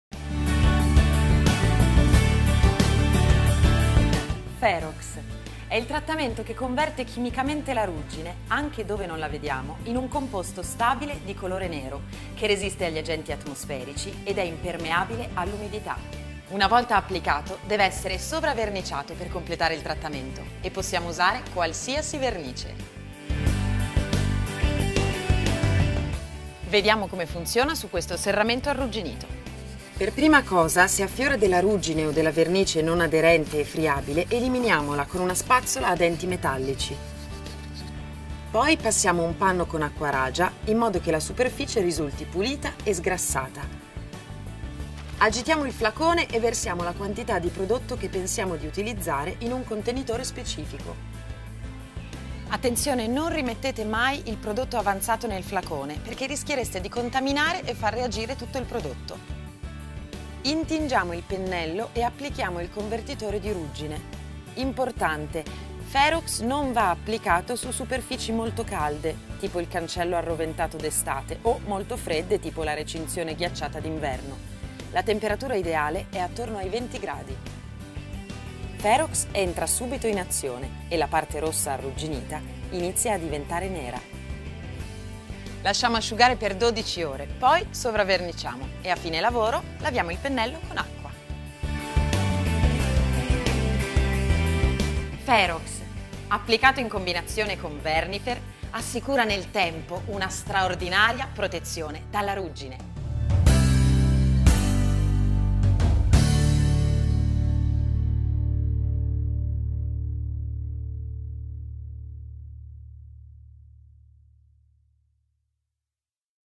FEROX - il converti ruggine
Prodotto per il trattamento della ruggine e per la protezione di tutte le superfici ferrose: arrugginite, nuove, verniciate e non.